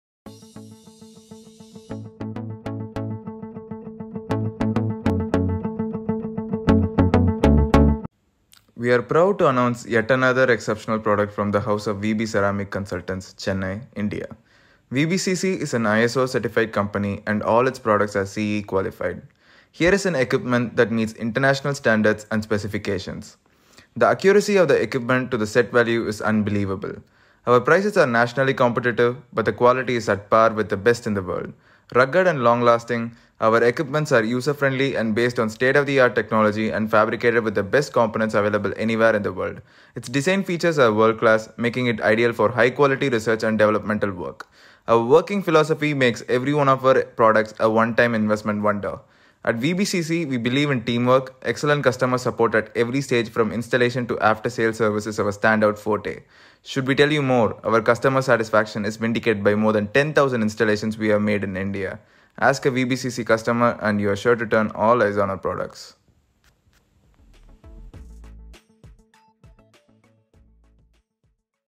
CVD - Computerised Chemical Vapor Disposition - Vertical type | VBCC
Today, we introduce Computerized Chemical Vapor Deposition (CVD) - Vertical Type, a cutting-edge solution for controlled materials deposition and thin film growth.

Designed for professionals, researchers, and industry pioneers, this CVD system offers state-of-the-art technology for a wide range of applications, from semiconductor manufacturing to materials science. Its precise control, automation features, and reliability make it an indispensable tool for various industries.

Join us as we delve into the capabilities of Computerized CVD - Vertical Type and discover how it can elevate your research, development, and production processes. If you're seeking precision, consistency, and the ability to deposit materials with efficiency, your quest ends here. Invest in Computerized CVD - Vertical Type and experience a new level of control and innovation in your work.